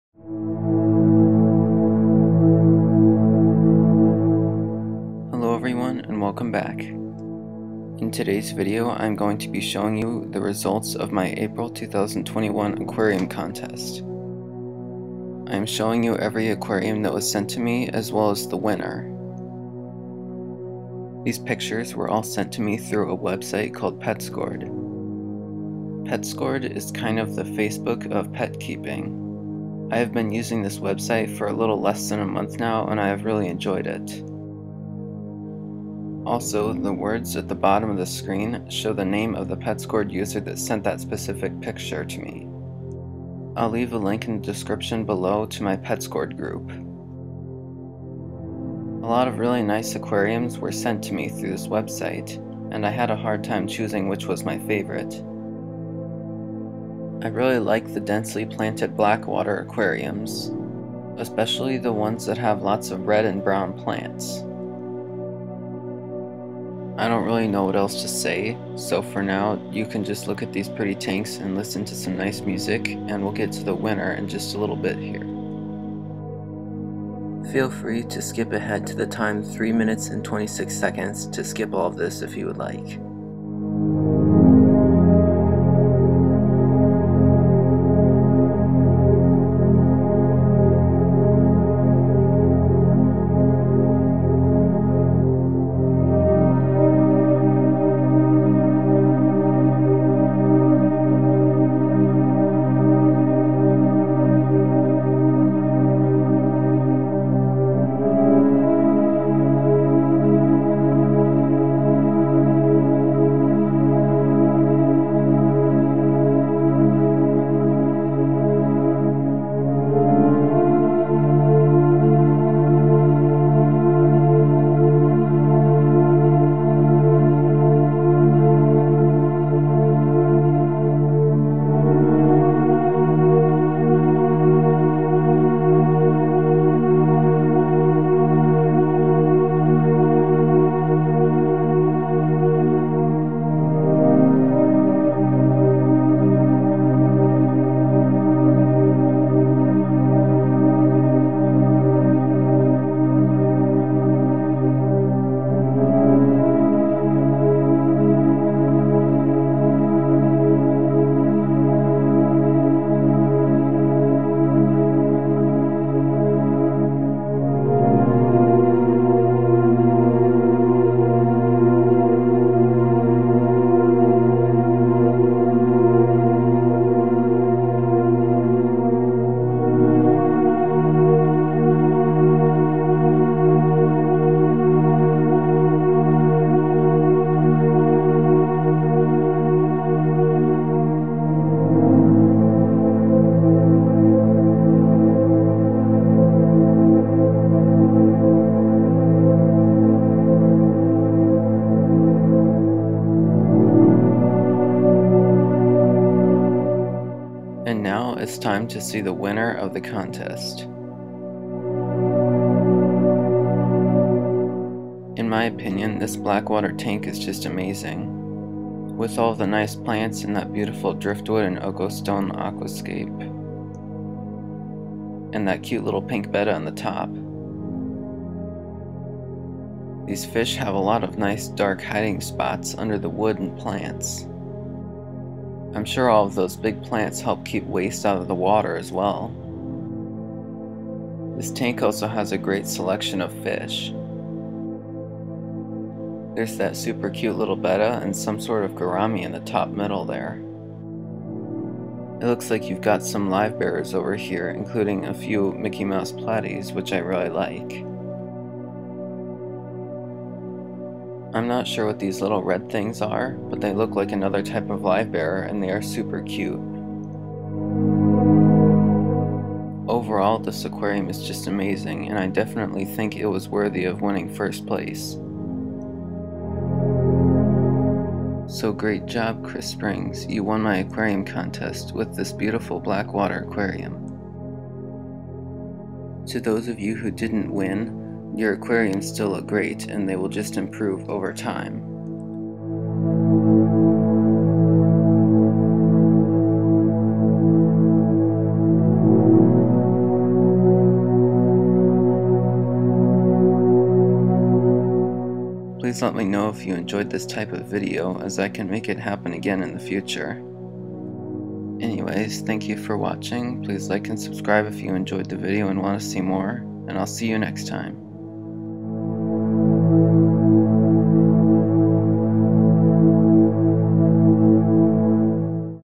April 2021 Aquarium Contest Results
Hi! In today's video, I am showing the results of my April 2021 aquarium contest. Though today is May 1, I originally made the Petscord post about the contest back in the middle of April.